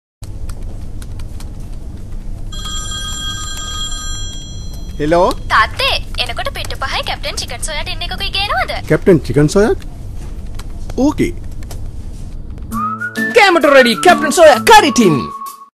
Captain Canned Soya TVC Boardroom
Producer | Chandana Meeriyagalla | Iranthi Abeysinghe (X10 Productions)
Agency | Leo Burnett
Agency Creatives | Ranjana Amaradeva | Malaka Samith
Agency Producer | Mohamed Ikram | Eranga Hemajith
Client Servicing | Sean Pompeous | Rinesh Jayaratne
DOP | Priyantha Malavige
Asst. Director | Primal Ranasinghe
Editor | Umesh Fernando
Colorist | Umesh Fernando
GFX | Madhuka Gunasekara | Chamishka Gamage
Music | Nisho Fernando
Art Director | Ranga Senanayake
Wardrobe | Janaka Ullundupitiya
Line Producer | Prasanna Kithalagama
Make Up | Priyantha Sirikumara
Model Coordination | Shag Yasitha (Yasitha Sankajaya Yapa Bandara)